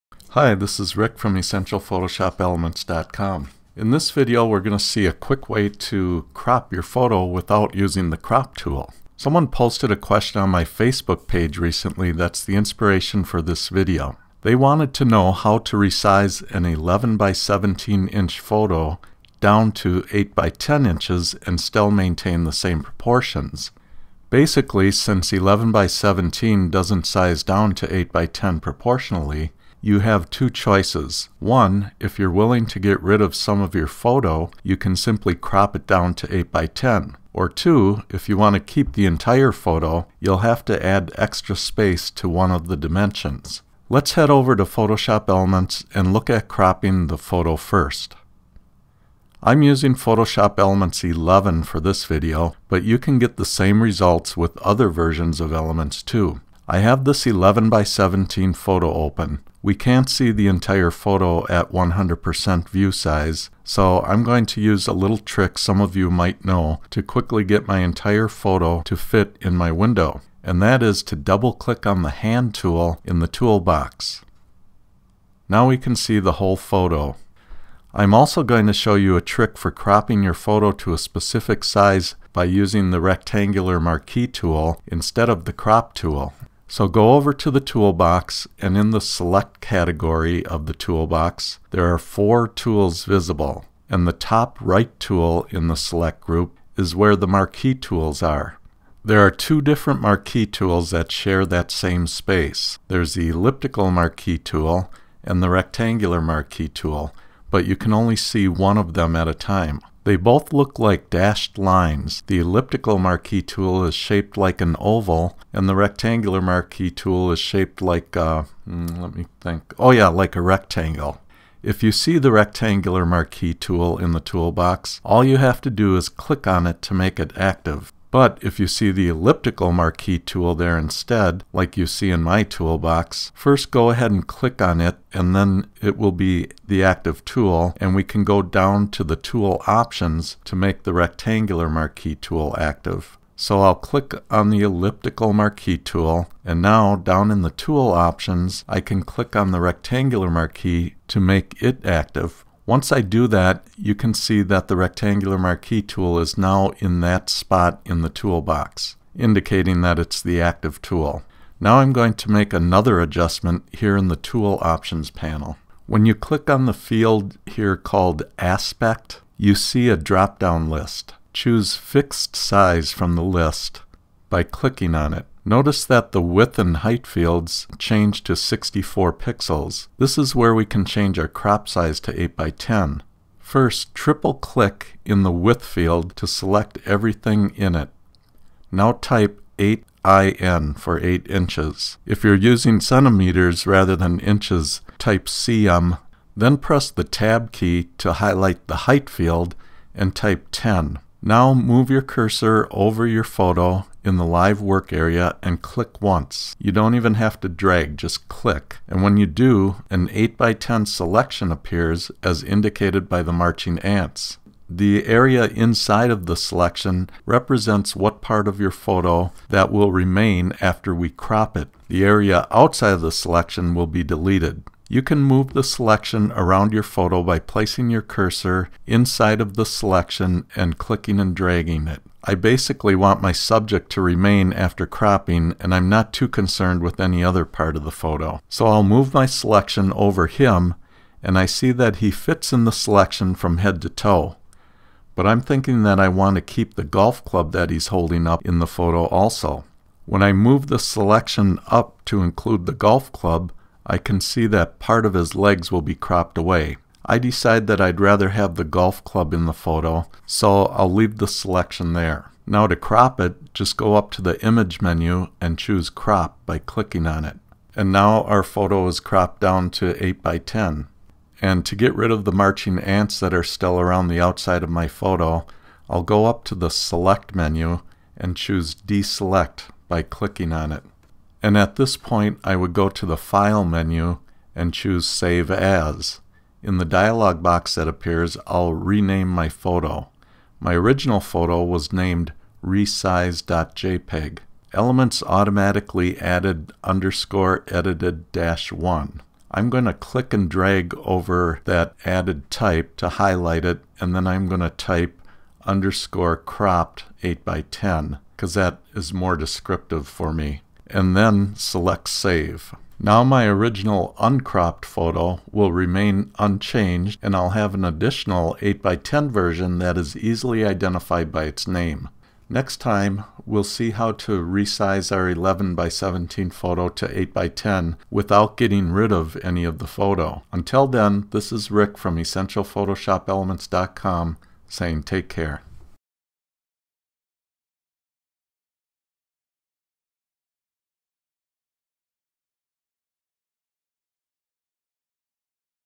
Photoshop Elements Can Quickly Crop Your Photo To Exact Size
Here's a secret technique to quickly crop your photos in Photoshop Elements without even using the Crop tool!!